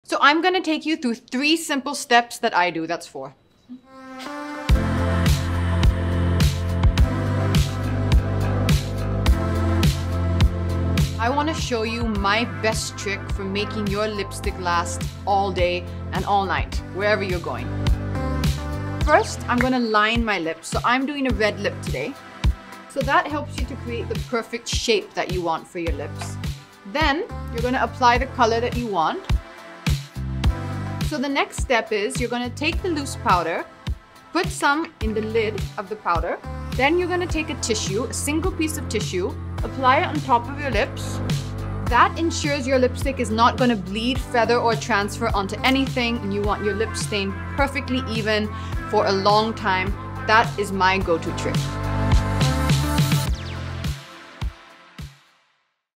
Get The Perfect Bold Lip | Katrina Kaif's Lipstick Hack | AskKay | Nykaa
No need to shy away from wearing bold lip colours anymore!! Katrina Kaif is here to show us her lipstick hack to make sure your bold lip stays put all day long 💋💄